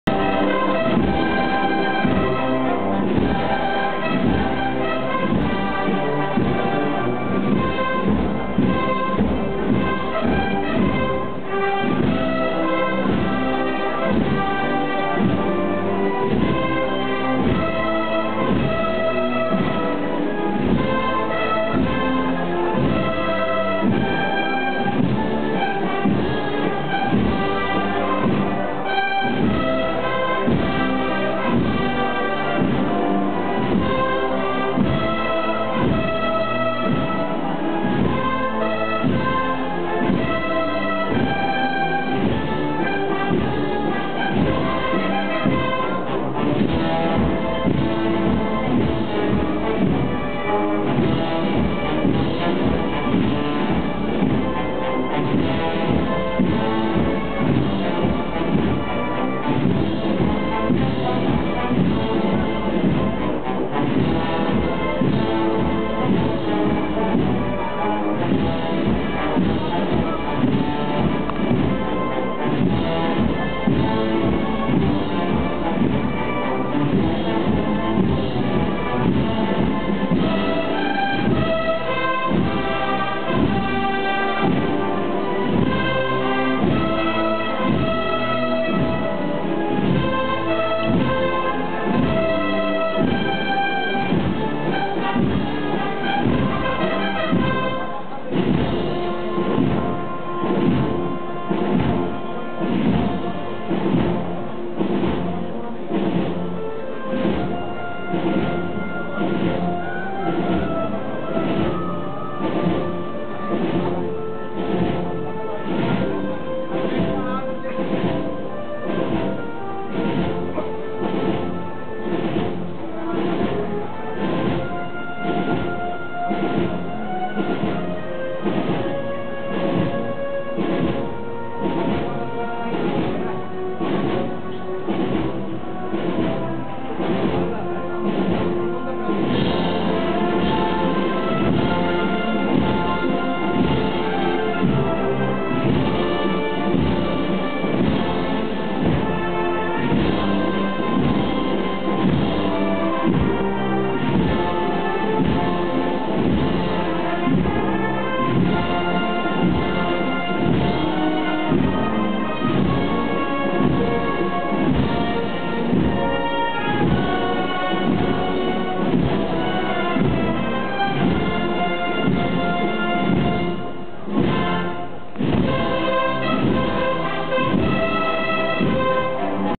Virgen de la Estrella en San Jacinto (2) 2012
Procesión de la Virgen de la Estrella, en San Jacinto, en la noche del Domingo de Ramos del 2012